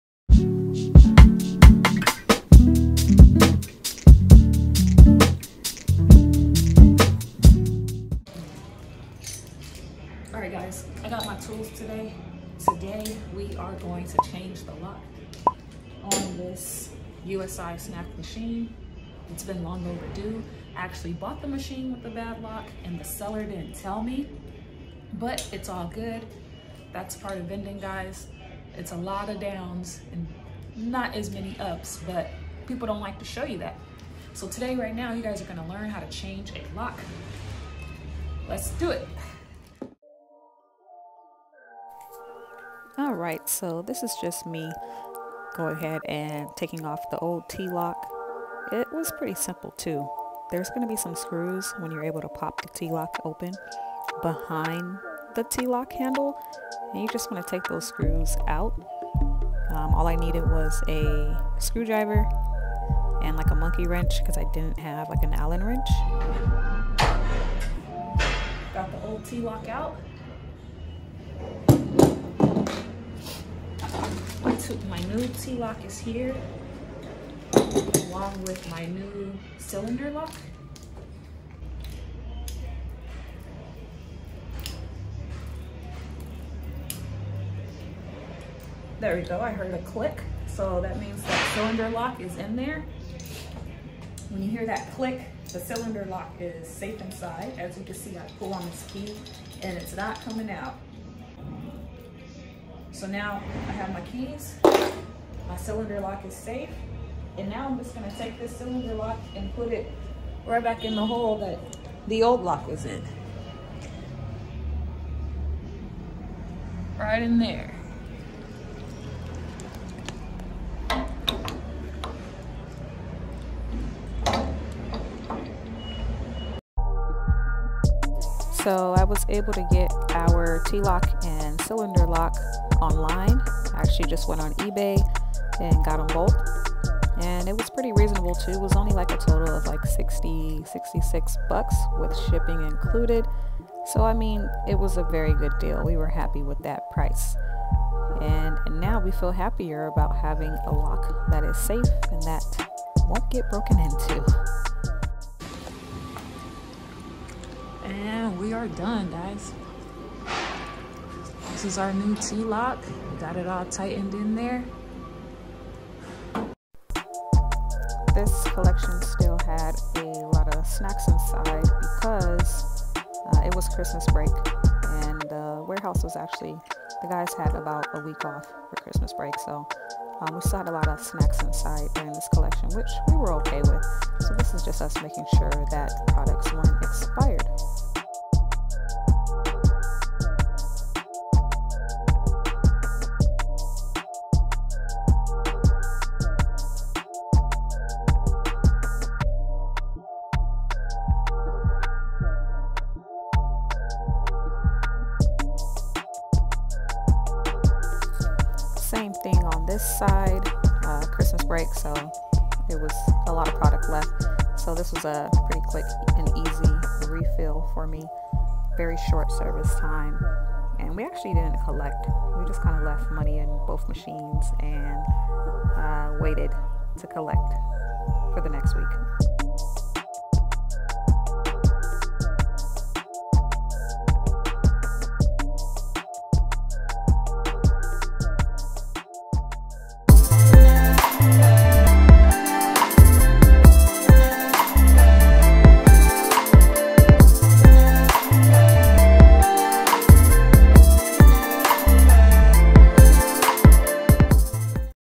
HOW TO REPLACE A VENDING MACHINE LOCK
In this video, we are changing our VENDING MACHINE LOCK to protect our assets, as well as servicing four of our snack and soda vending machines. To change a vending machine lock you will need to have a new cylinder lock (required), and a new t-handle (optional). You can find both parts on eBay for an extremely reasonable price. Thanks for rocking with us!

⭐️BONUS: Get a discount on your very own Nayax Credit Card Reader by visiting:

Use Code: Rayzo

Use Code: Rayzo